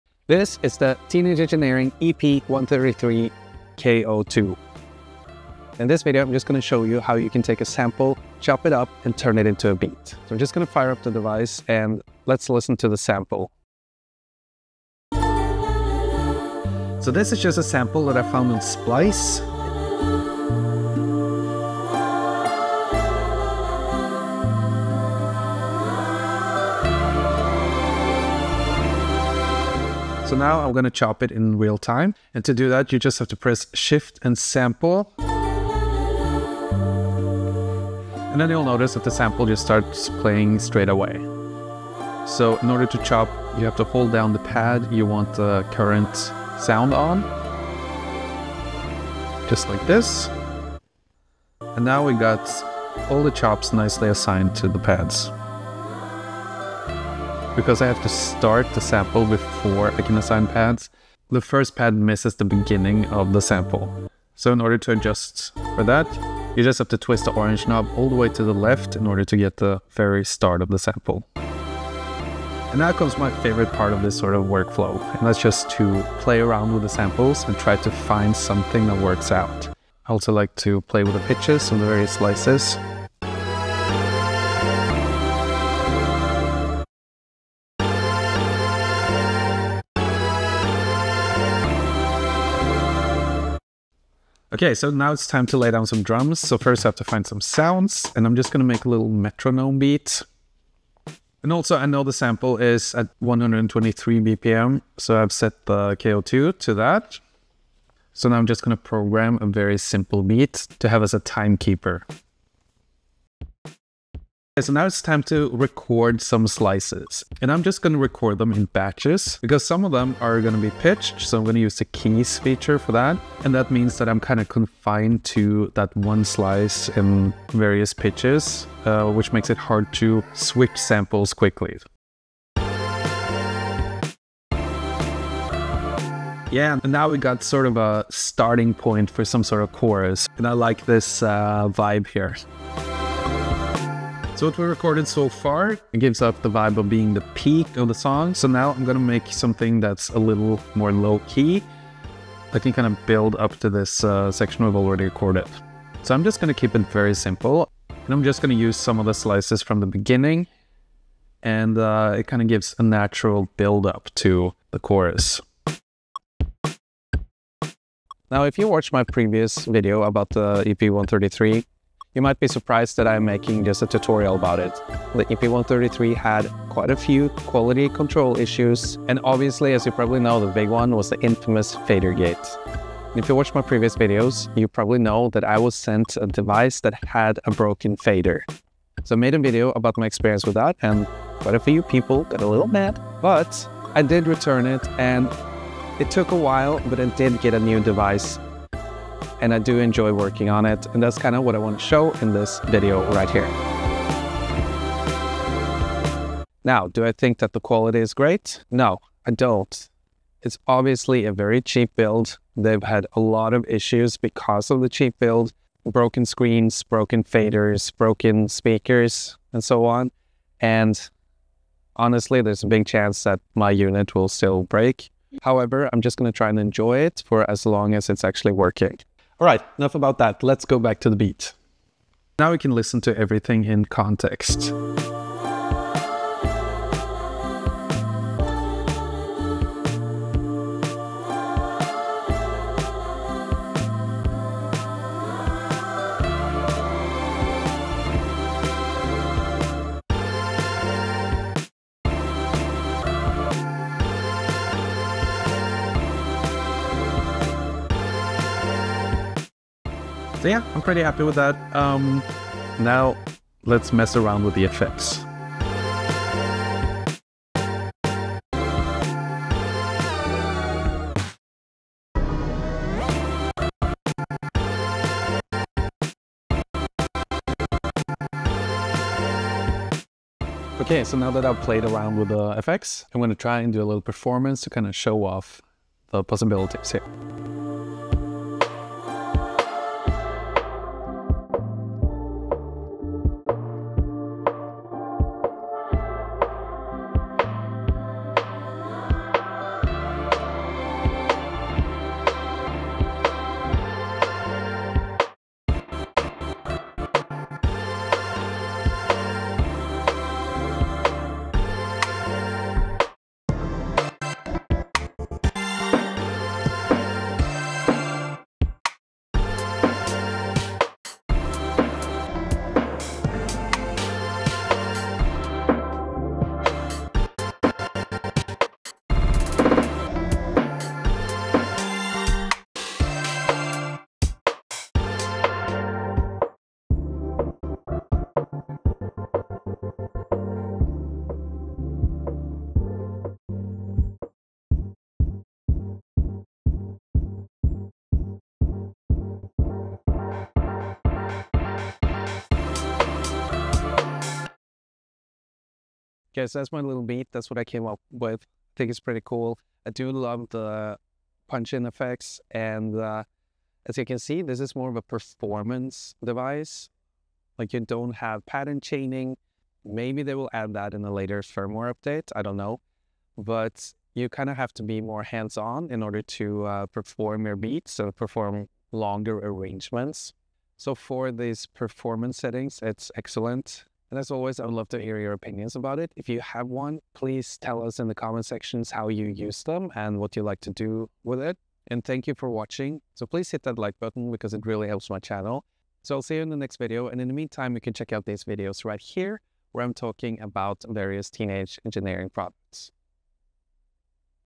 Did I change my mind with a functioning KO2? Slicing samples on the EP-133
I made a video about my broken EP-133 device, where I was the unlucky recipient of a fadergate device. And I was not merciful, however, now I finally have a new device, and in this video I show you how you can slice samples in order to create a beat.

00:00 - intro
00:11 - slicing the sample
01:11 - playing with the slices
01:34 - laying down a metronome beat
01:53 - recording
02:47 - the quality problem
04:19 - playing with fx
04:40 - performance and conclusion